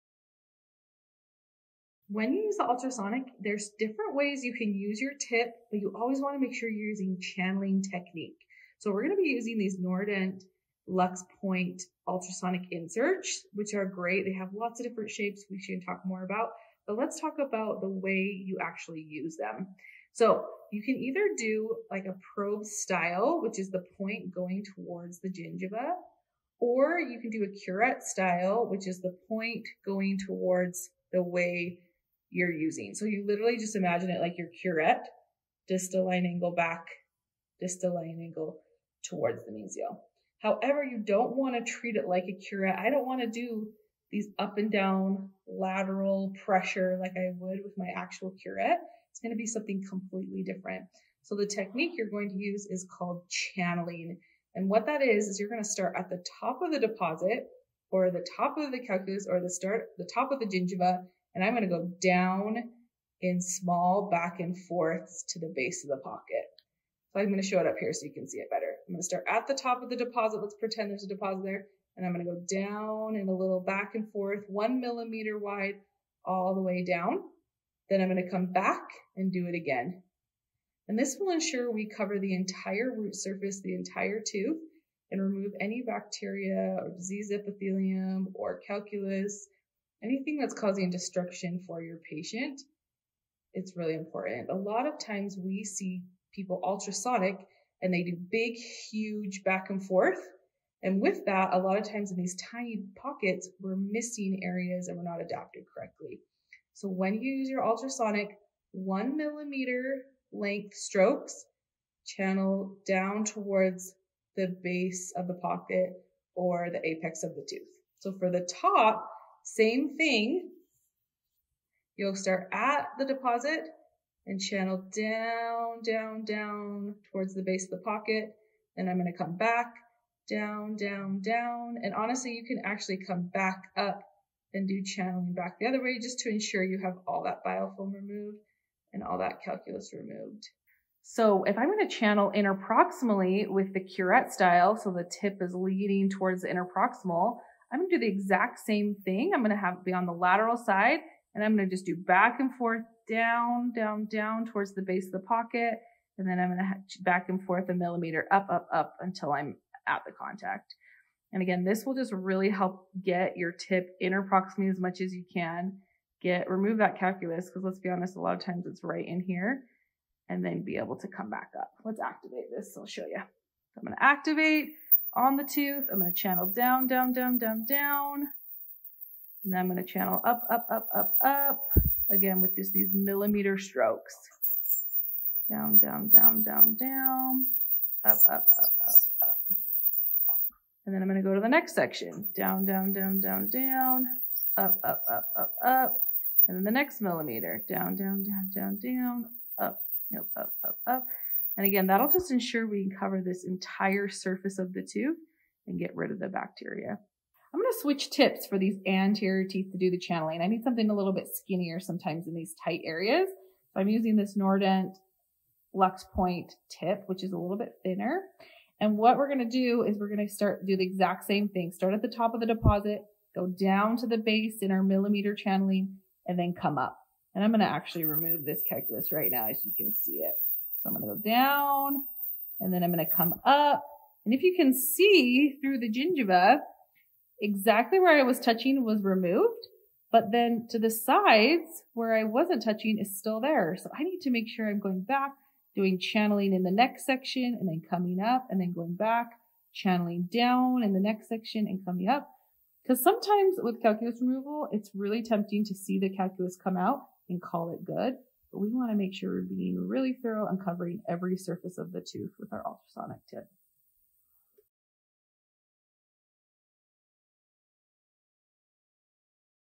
How to Channel with an Ultrasonic Tip Featuring Nordent LuxPoint
Ultrasonics have definitely changed the way we do dental hygiene, but are we using them correctly? Find out how to use a channeling technique to be the most
effective when it comes to calculus and biofilm removal.
A huge thank you to Nordent Manufacturing for supporting us at Hygiene Edge and
all dental professionals by creating high quality instruments and magnetostrictive
inserts like LuxPoint! To learn more about these instruments and getting them